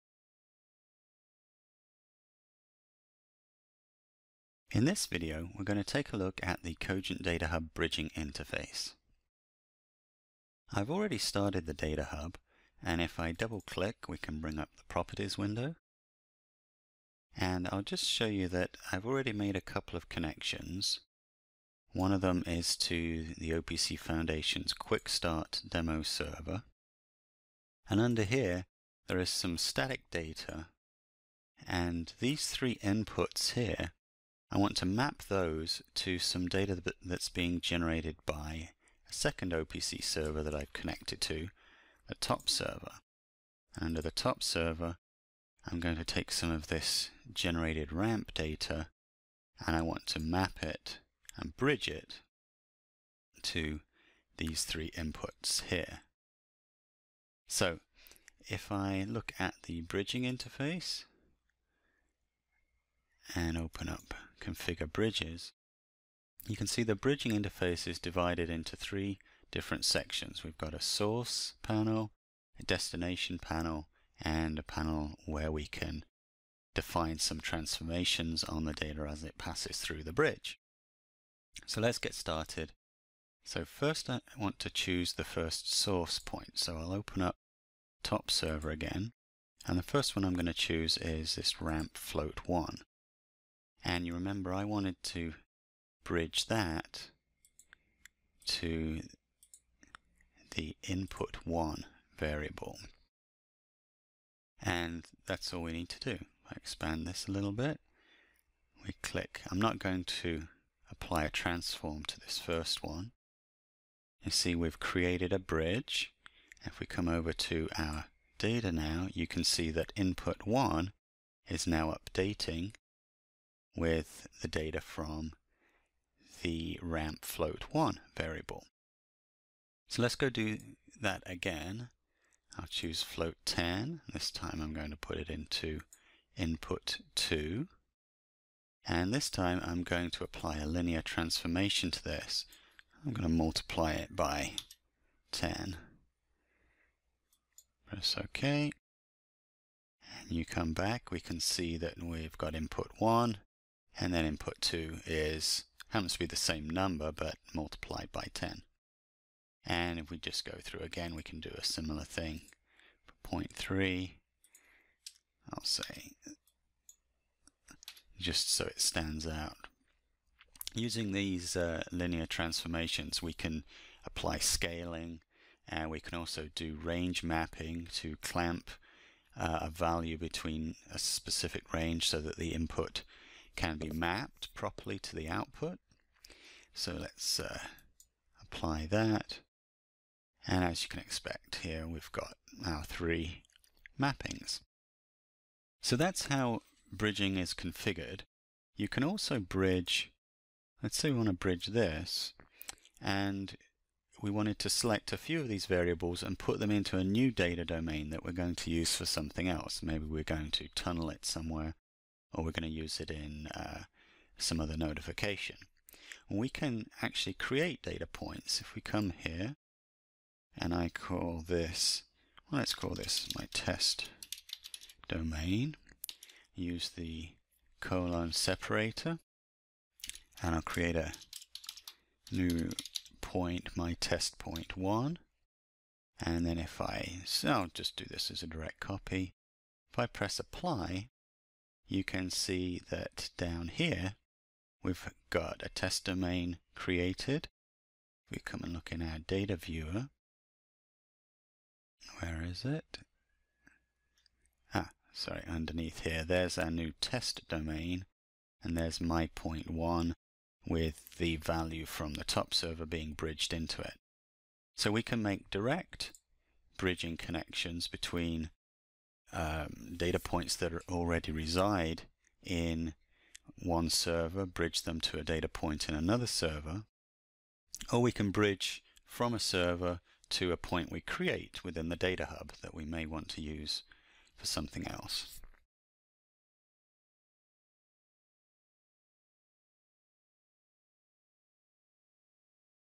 Configuring Point-to-Point Bridging in Cogent DataHub
A Cogent DataHub "How-To" video that walks you through how to configure the bridging interface.